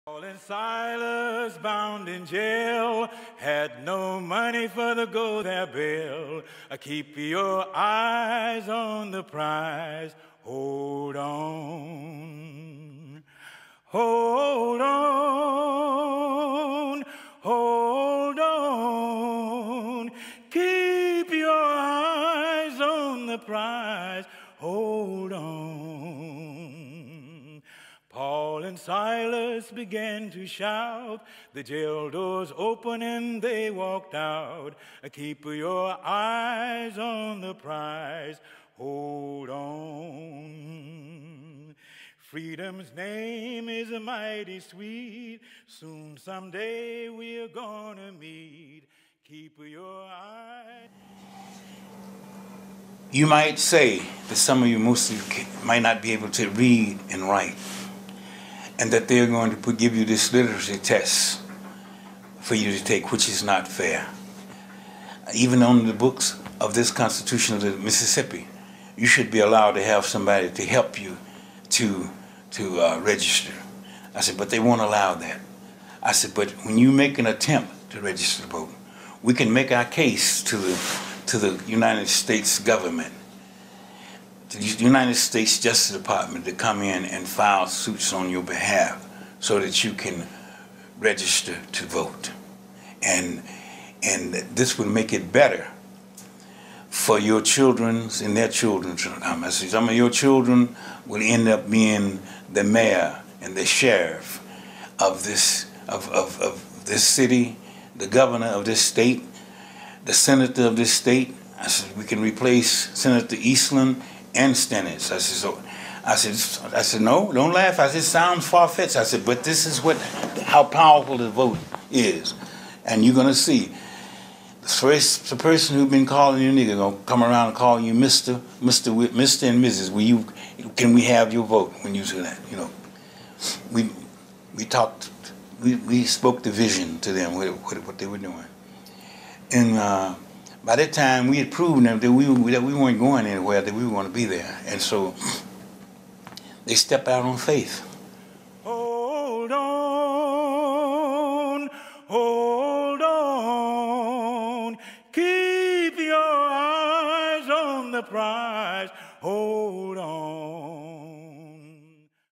Willie B Wazir Peacock, Stand for Freedom, We Spoke the Vision to Them
A civil rights organizer recorded a 3 hour video interview with us for his legacy.
The moving video is in 3 Civil Rights libraries in the Country and has been viewed more than 2,000 times.